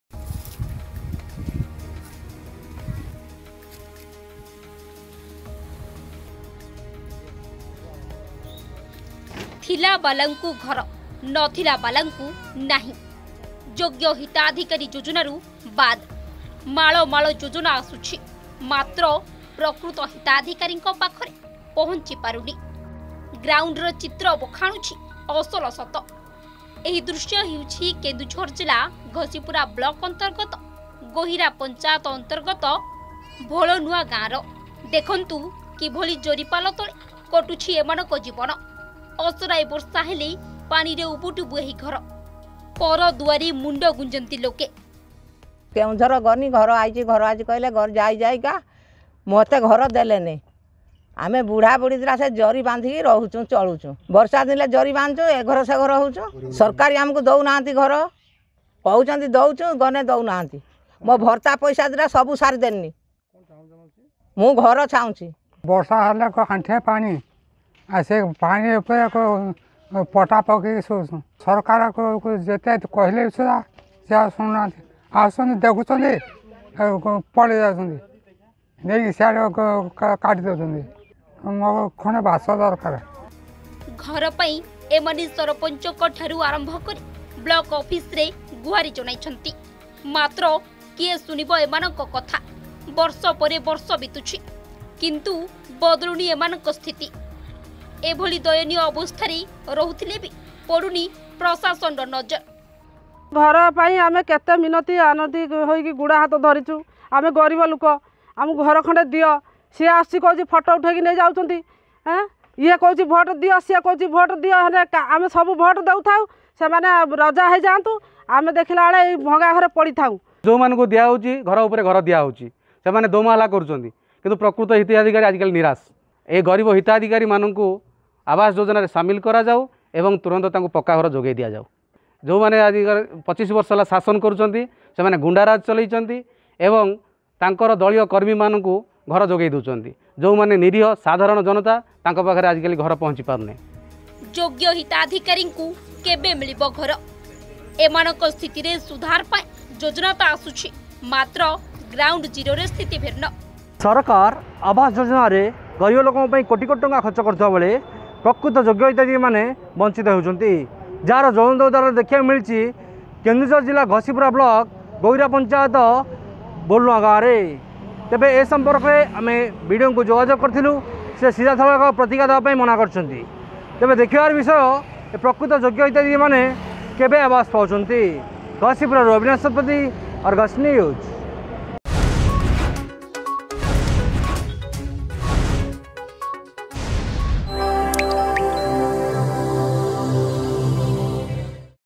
ଥିଲା ବାଲାଙ୍କୁ ଘର, ନଥିଲା ବାଲାଙ୍କୁ ନାହିଁ
ମୁଣ୍ଡ ଉପରେ ଚାଳ ନାହିଁ। ପଲିଥିନ ତଳେ କଟୁଛି ଜୀବନ। ବର୍ଷା ହେଲେ ଘରେ ପଶୁଛି ପାଣି। କେନ୍ଦୁଝର ଜିଲ୍ଲା ଘସିପୁରା ବ୍ଲକରେ ଆବାସ ଯୋଜନାରେ ବାସ୍ତବ ସତ ଦେଖିବାକୁ ମିଳିଛି।